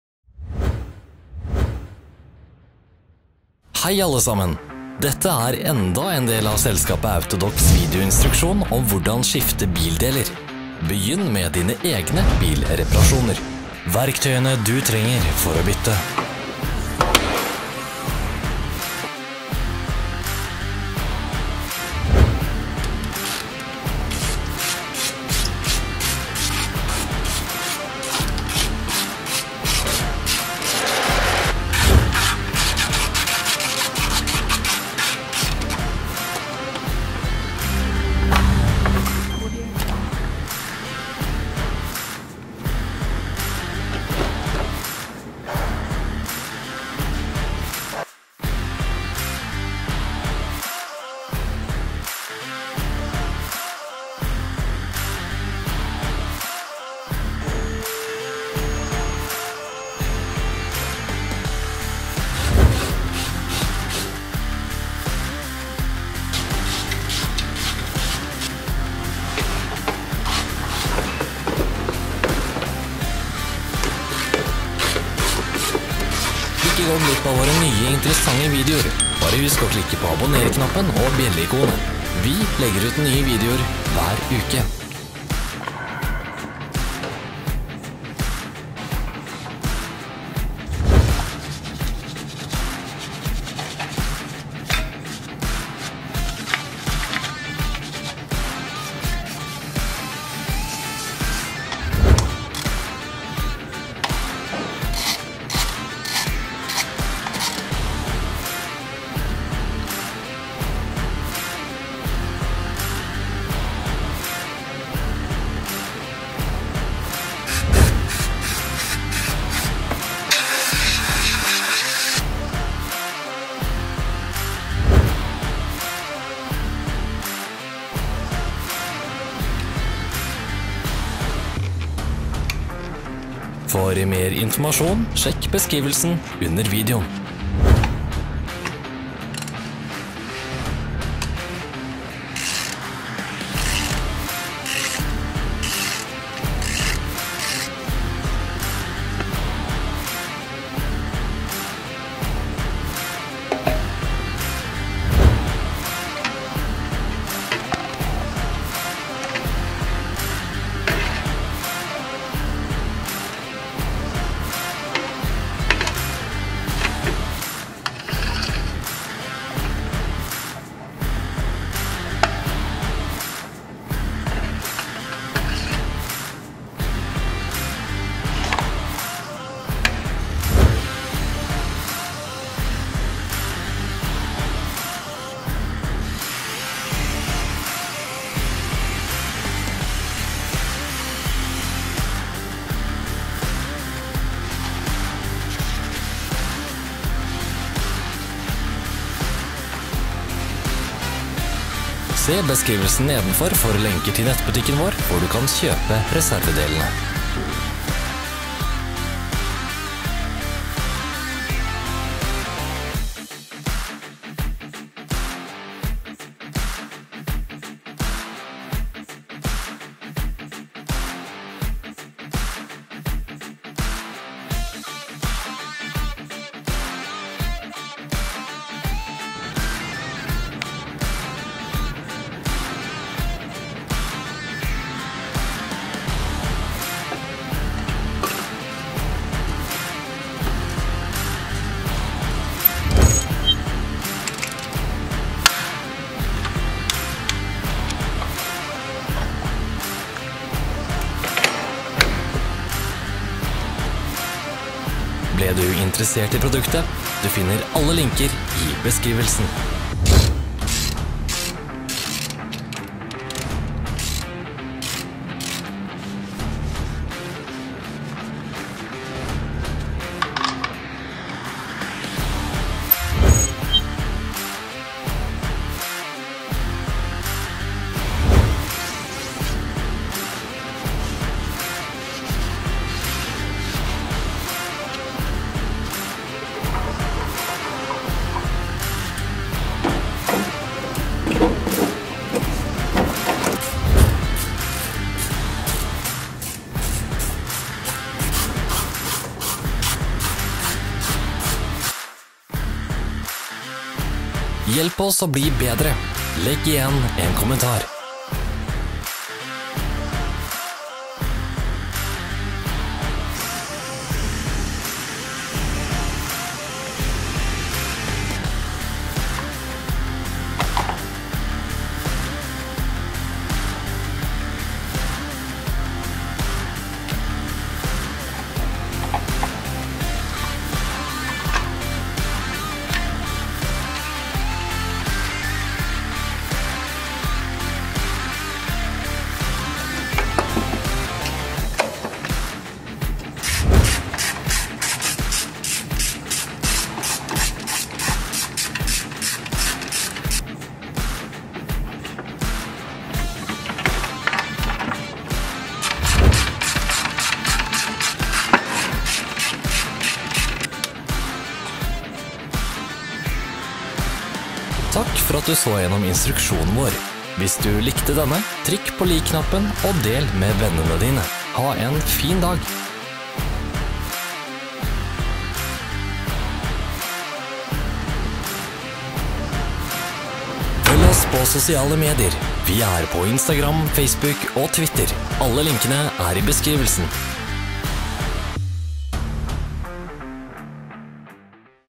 Hvordan bytte oljefilter og motorolje der på MERCEDES-BENZ W211 E-Klasse [AUTODOE-VIDEOLEKSJONER]
Hvordan bytte oljefilter og motorolje på MERCEDES-BENZ E270 3 (W211) 2.7 Sedan 2002–2009 [AUTODOE-VIDEOLEKSJONER]

Oljefilter og motorolje til kjøretøyet MERCEDES-BENZ CLS Coupe (C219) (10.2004–12.2010) / MERCEDES-BENZ E-Class T-modell (S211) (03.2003–07.2009) / MERCEDES-BENZ E-Class Saloon (W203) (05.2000–02.2007) / MERCEDES-BENZ S-Class Saloon (W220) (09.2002–08.2005) bør som regel erstattes i én og samme prosedyre. Det er imidlertid noen små forskjeller, på grunn av spesifikke egenskaper ved kjøretøyets design.

Alle video- og PDF-veiledninger om utskifting av deleri MERCEDES-BENZ W211: last ned og se på AUTODOC CLUB

0:00 — MERCEDES-BENZ W211 E-Klasse – oljefilter og motorolje: trinn-for-trinn veiledning for utskifting
1:03 — Løsne festene på det nedre dekselet til oljepannen.
1:42 — Skru ut bunnpluggen.
3:25 — Installer nye O-ringer til filterdekselets monteringsfester.
4:02 — Skru inn den nye dreneringspluggen og stram den.
5:02 — Tøm oljen inn i motoren gjennom påfyllingshullet.